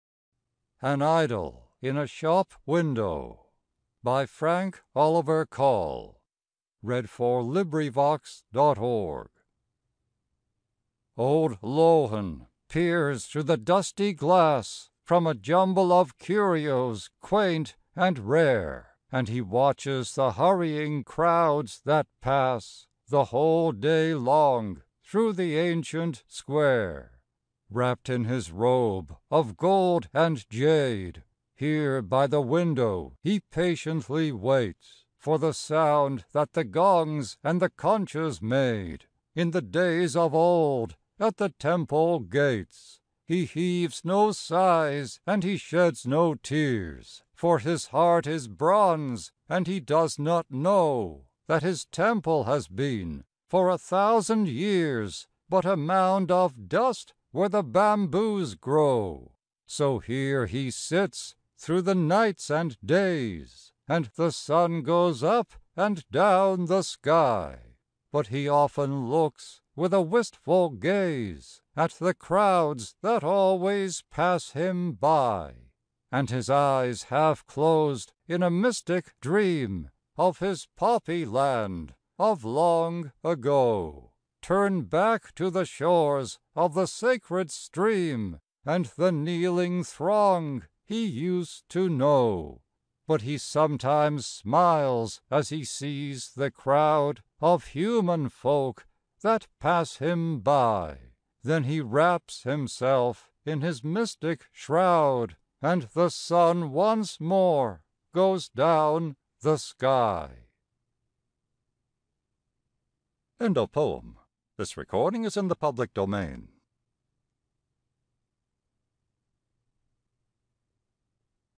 Frank Oliver Call - In A Belgian Garden And Other Poems: An Idol In A Shop Window
IN A BELGIAN GARDEN AND OTHER POEMS.
These magnificent poems written by a lover of the natural splendor of untrodden lands are both thrilling and exhilarating. Visions and observations of the wonders of creation are gradually unlocked and elegantly illustrated in ways hitherto unimagined by the passive observer of the eclectic world that surrounds us, while sparing no omission of the very obvious and egregious rampant destruction, both physical and moral, of the Great War.

AUTHOR BIOGRAPHY.
Frank Oliver Call (April 11, 1878 – September 7, 1956) was a Canadian poet and academic. Born in Brome Lake, Quebec, Call was educated at Bishop's University in Paris and Marburg and at McGill University, and was subsequently a professor of languages at Bishop's and McGill.

His publications as a poet included In a Belgian Garden (1916), Acanthus and Wild Grape (1920), Blue Homespun (1924) and Sonnets for Youth (1944). Acanthus and Wild Grape, his most famous work, was divided in two sections: Acanthus followed more traditional Victorian poetic styles, while Wild Grape was written as free verse. As a result of that work, Call is seen as a bridge between early Canadian poets such as Bliss Carman, Archibald Lampman and Duncan Campbell Scott, and the modernist work of later poets such as E. J. Pratt and Dorothy Livesay. More recent analysis has also concentrated on homoerotic themes in some of his writing, particularly in Sonnets for Youth, although there is not currently sufficient biographical evidence to confirm whether Call ever actually identified as gay.

In addition to writing poetry, Call published two volumes of travel writing, The Spell of French Canada (1926) and The Spell of Acadia (1930), and a biography of Marguerite Bourgeoys. Call won the Quebec Literary Competition Award in 1924 for Blue Homespun. In addition, he was involved in Canadian Poetry Magazine, the Canadian Authors Association and PEN Canada.